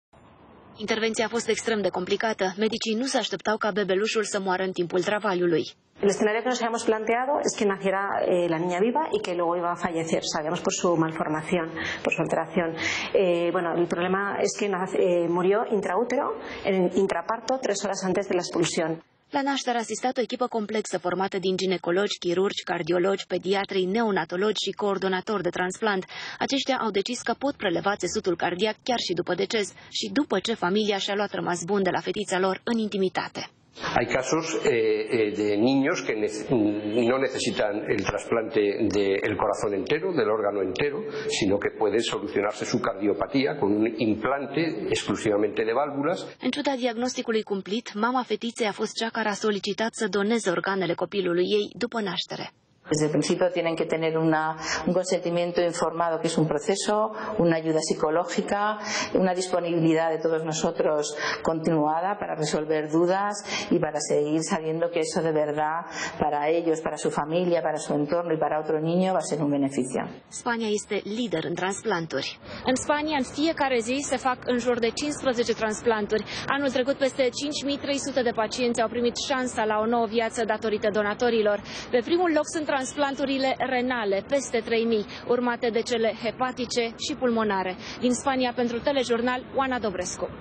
Premieră medicală în Spania
Medicii din Spania au reușit transplantarea valvelor cardiace prelevate de la un bebeluș care a murit în timpul nașterii. Familia știa că fetița suferă malformații grave și că nu va supraviețui, dar mama a hotărât să ducă sarcina la capăt pentru a salva viața altor copii care așteaptă un donator.